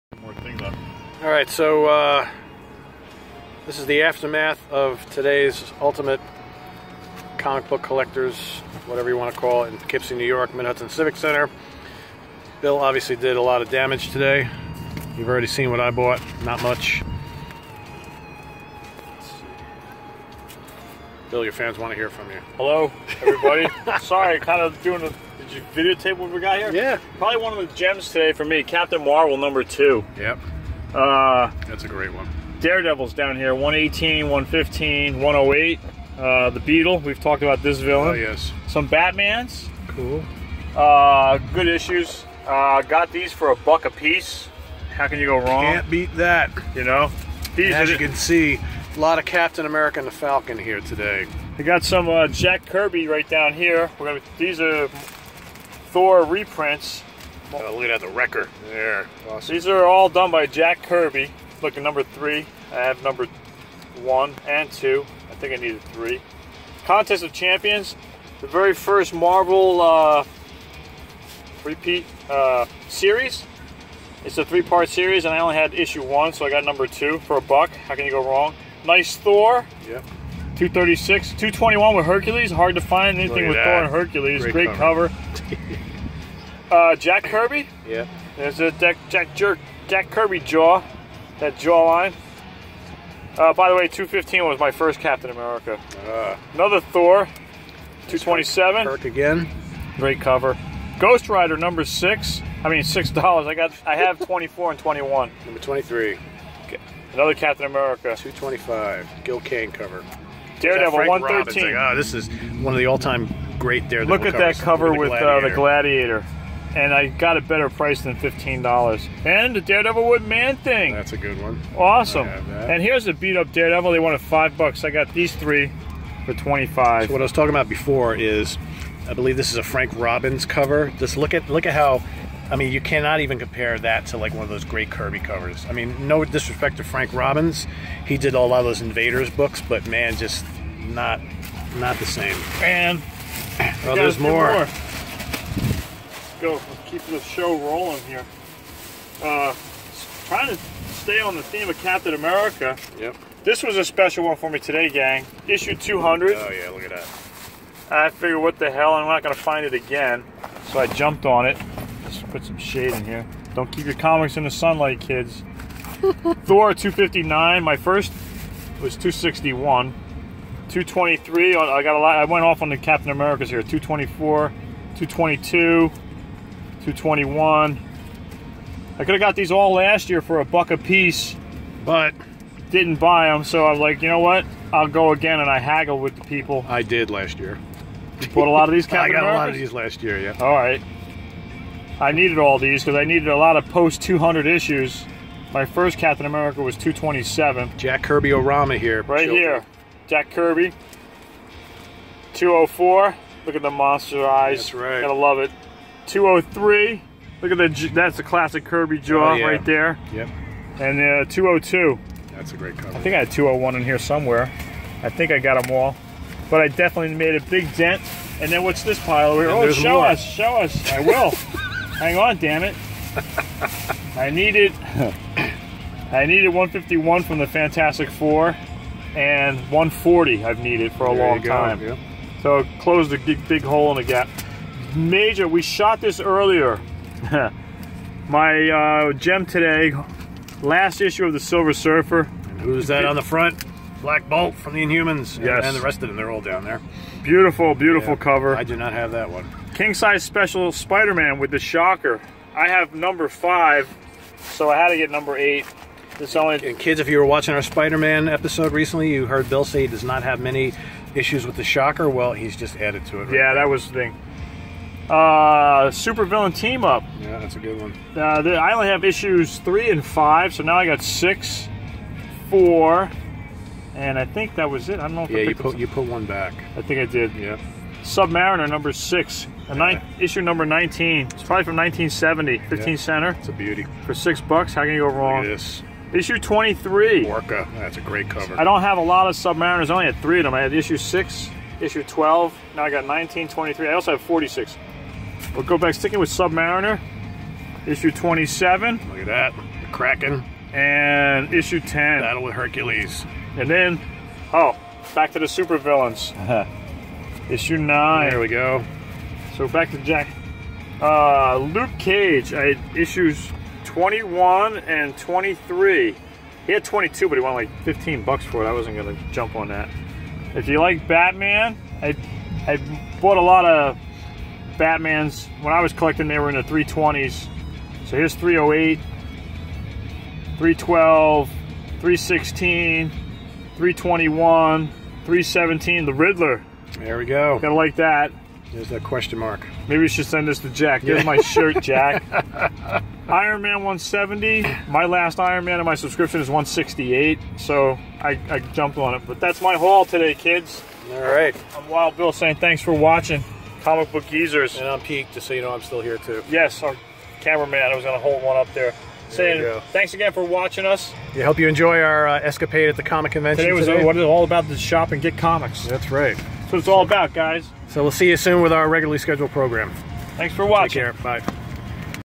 Comic Book Geezers @ Ultimate Comic Convention Part 7
Join Comic Book Geezers Wild Bill & Pete as they invade the Ultimate Comic Book Convention in Poughkeepsie, NY on 9/10/2017!